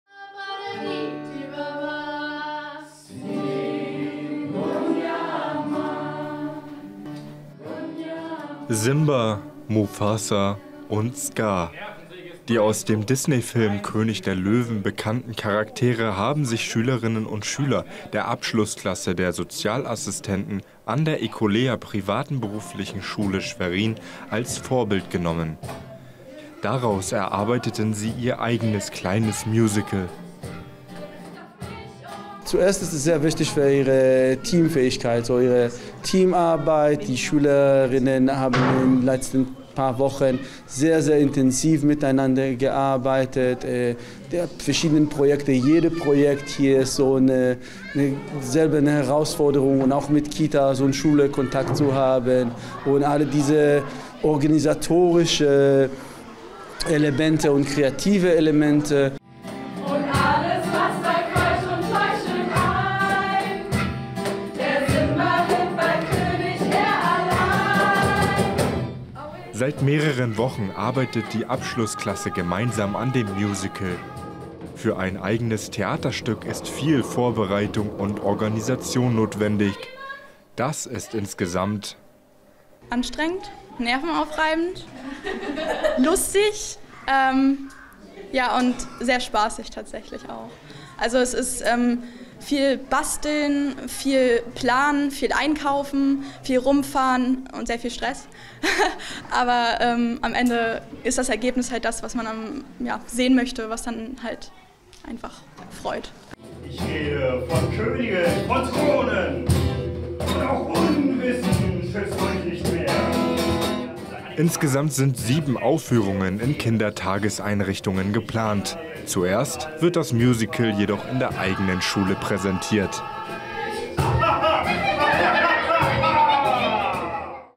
Simba, Mufasa und Scar proben an der ecolea | Private Berufliche Schule Schwerin
Die Abschlussklasse der Sozialassistenten an der ecolea | Private Berufliche Schule Schwerin hat  am 8. Juni eine Generalprobe ihres Theaterstücks durchgeführt. Inspiriert wurden sie von dem Disney-Film König der Löwen. Die Hauptcharaktere Simba, Scar und Mufasa sowie viele weitere tierische Rollen übernahmen die Schülerinnen und Schüler.